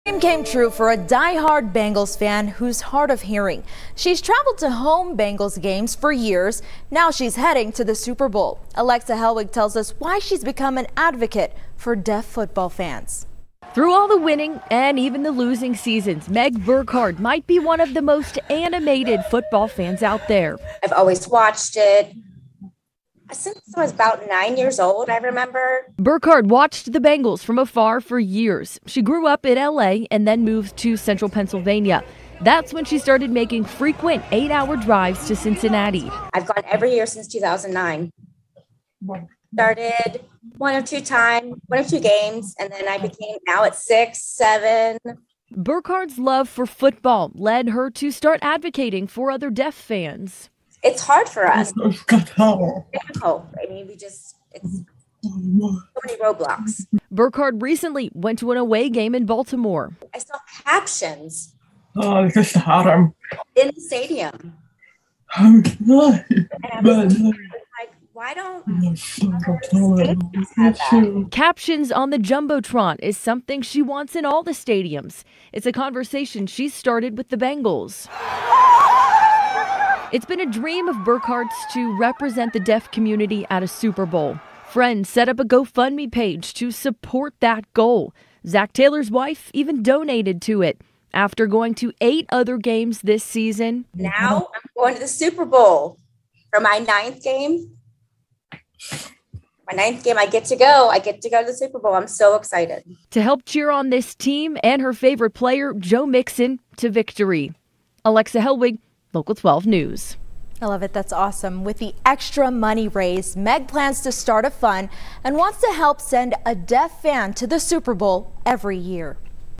Deaf Bengals fan going to Super Bowl advocates for other deaf NFL fans
A dream came true for a die-hard Bengals fan who's deaf